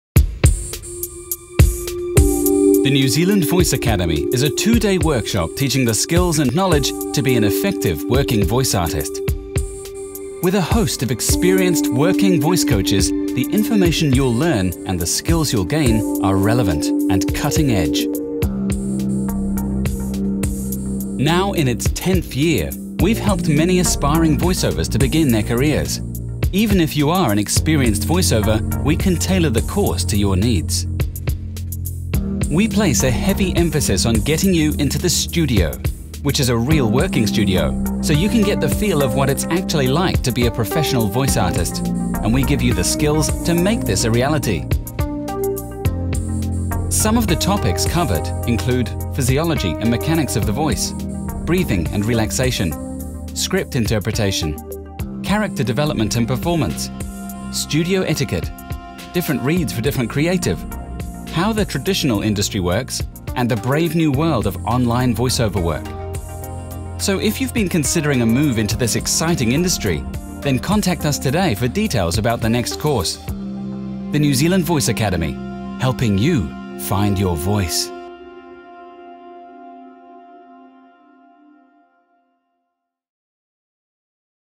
The New Zealand Voice Academy
Learn how to be a professional voice over voice artist by attending the only academy of it's type in New Zealand. The New Zealand Voice Academy. This 2 day workshop gives you the skills and knowledge to become a professional voice artist.